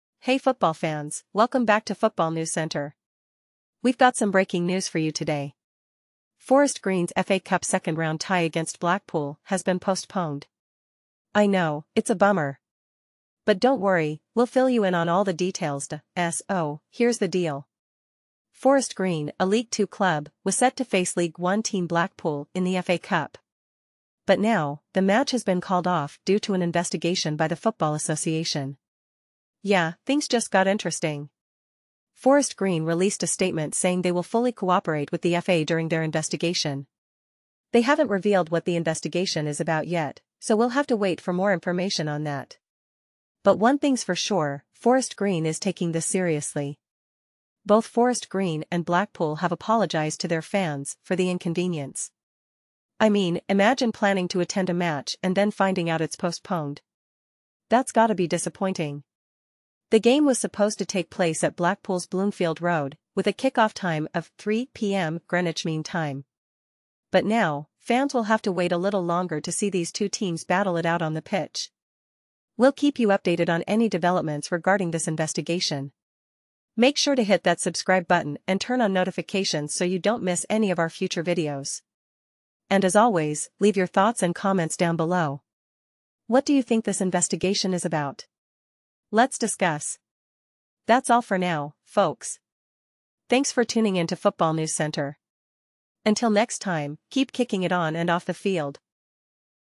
Forest Green FA Cup tie off amid FA investigation
The FA Cup second-round tie between Forest Green and Blackpool has been postponed due to an investigation by the Football Association (FA) into Forest Green. The exact nature of the investigation has not been disclosed. Forest Green released a statement saying they would cooperate with the FA and update fans accordingly. The match was scheduled to take place at Blackpool's Bloomfield Road stadium. Both Forest Green and Blackpool issued apologies to fans for the inconvenience caused by the postponement.